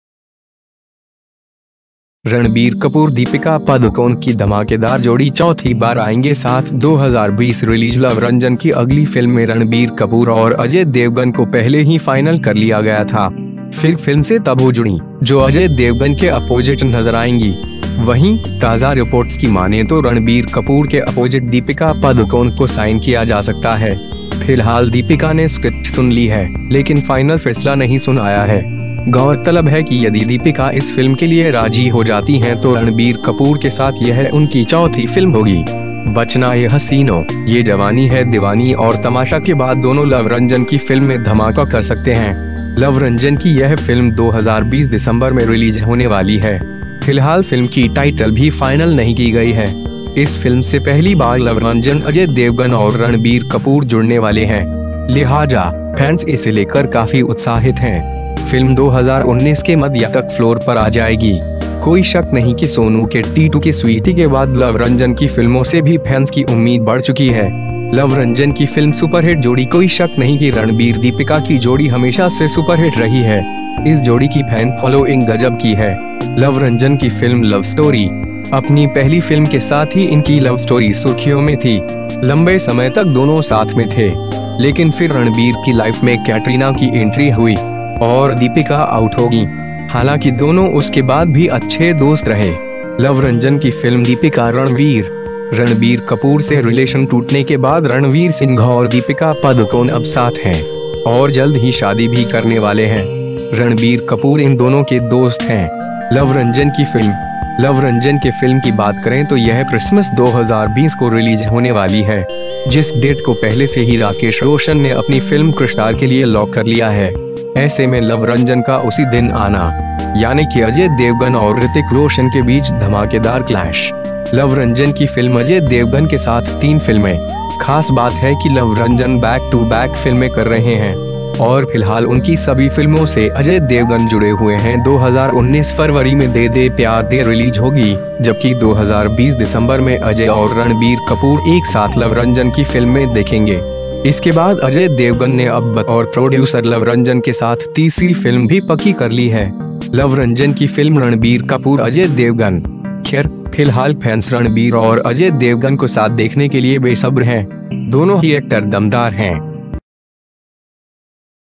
रणबीर कपूर दीपिका पादुकोण की धमाकेदार जोड़ी चौथी बार आएंगे साथ 2020 रिलीज
लव रंजन की अगली फिल्म में रणबीर कपूर और अजय देवगन को पहले ही फाइनल कर लिया गया था। फिर फिल्म से तबू जुड़ीं, जो अजय देवगन के अपोजिट नजर आएंगी। वहीं, ताजा रिपोर्ट्स की मानें तो रणबीर कपूर के अपोजिट दीपिका पादुकोण को साइन किया जा सकता है। फिलहाल दीपिका ने स्क्रिप्ट सुन ली है, लेकिन फाइनल फैसला नहीं सुनाया है।

गौरतलब है कि यदि दीपिका इस फिल्म के लिए राजी हो जाती हैं तो रणबीर कपूर के साथ यह उनकी चौथी फिल्म होगी। बचना ए हसीनो, ये जवानी है दिवानी और तमाशा के बाद दोनों लव रंजन की फिल्म में धमाका कर सकते हैं। लव रंजन की यह फिल्म 2020 दिसंबर में रिलीज होने वाली है।

फिलहाल फिल्म की टाइटल भी फाइनल नहीं की गई है। इस फिल्म से पहली बार लव रंजन- अजय देवगन और रणबीर कपूर जुड़ने वाले हैं। लिहाजा, फैंस इसे लेकर काफी उत्साहित हैं। फिल्म 2019 के मध्य तक फ्लोर पर आ जाएगी। कोई शक नहीं कि सोनू के टीटू की स्वीटी के बाद लव रंजन की फिल्मों से भी फैंस की उम्मीद बढ़ चुकी है।

लव रंजन की फिल्म
सुपरहिट जोड़ी
कोई शक नहीं कि रणबीर- दीपिका की जोड़ी हमेशा से सुपरहिट रही है। इस जोड़ी की फैन फॉलोइंग गजब की है।

लव स्टोरी

अपनी पहली फिल्म के साथ ही इनकी लव स्टोरी सुर्खियों में थी। लंबे समय तक दोनों साथ में थे। लेकिन फिर रणबीर की लाइफ में कैटरीना की एंट्री हुई.. और दीपिका आउट हो गईं। हालांकि दोनों उसके बाद भी अच्छे दोस्त रहे।

दीपिका- रणवीर

रणबीर कपूर से रिलेशन टूटने के बाद रणवीर सिंह और दीपिका पादुकोण अब साथ हैं.. और जल्द ही शादी भी करने वाले हैं। रणबीर कपूर इन दोनों के दोस्त हैं।

लव रंजन के फिल्म की बात करें तो यह क्रिसमस 2020 को रिलीज होने वाली है। जिस डेट को पहले से ही राकेश रोशन ने अपनी फिल्म कृष 4 के लिए लॉक कर लिया है। ऐसे में लव रंजन का उसी दिन आना.. यानि की अजय देवगन और ऋतिक रोशन के बीच धमाकेदार क्लैश।

अजय देवगन के साथ तीन फिल्में

खास बात है कि लव रंजन बैक टू बैक फिल्में कर रहे हैं। और फिलहाल उनकी सभी फिल्मों से अजय देवगन जुड़े हुए हैं। 2019 फरवरी में 'दे दे प्यार दे' रिलीज होगी, जबकि 2020 दिसंबर में अजय और रणबीर कपूर एक साथ लव रंजन की फिल्म में दिखेंगे। इसके बाद अजय देवगन ने अब बतौर प्रोड्यूसर लव रंजन के साथ तीसरी फिल्म भी पक्की कर ली है।

रणबीर कपूर- अजय देवगन

खैर, फिलहाल फैंस रणबीर और अजय देवगन को साथ देखने के लिए बेसब्र हैं। दोनों ही एक्टर दमदार हैं।